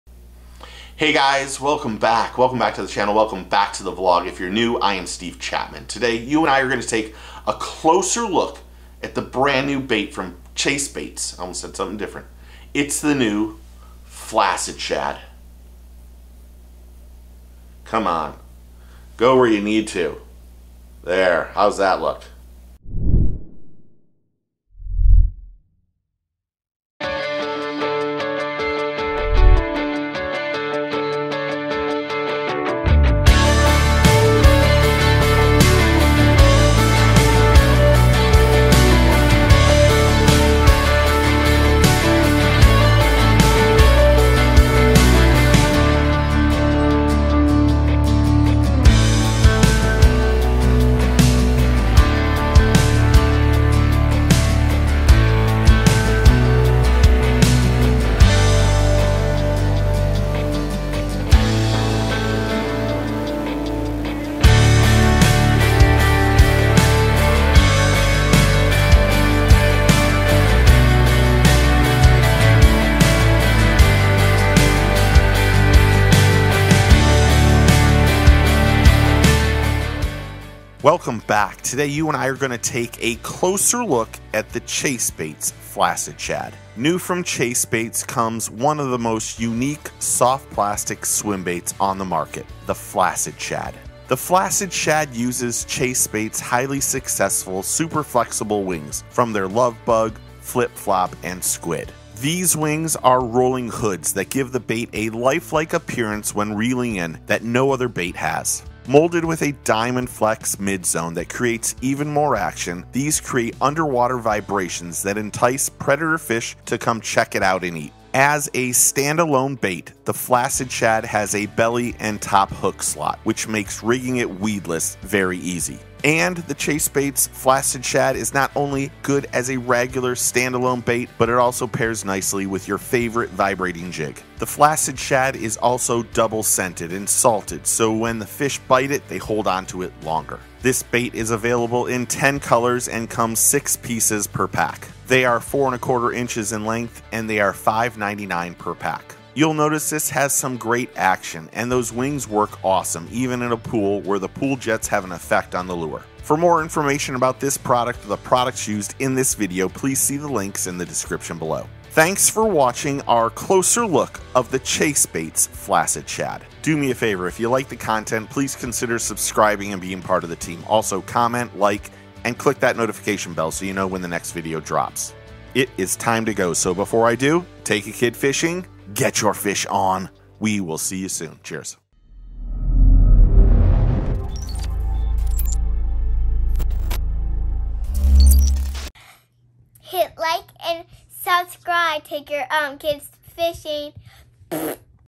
Closer Look at the Chasebaits Flacid Shad Largemouth Bass Fishing Swimbait
New from Chasebaits is the Flacid Shad largemouth bass fishing swim bait that uses a different hood to make this swimbait look more realistic and give it more action in the water. The Flacid Shad from Chasebaits in one of the most unique swim baits on the bass fishing market. The wings on the back of the Chasebaits Flacid Shad give the bait an awesome rolling appearance and give off water displacement to attract those targeted predator fish.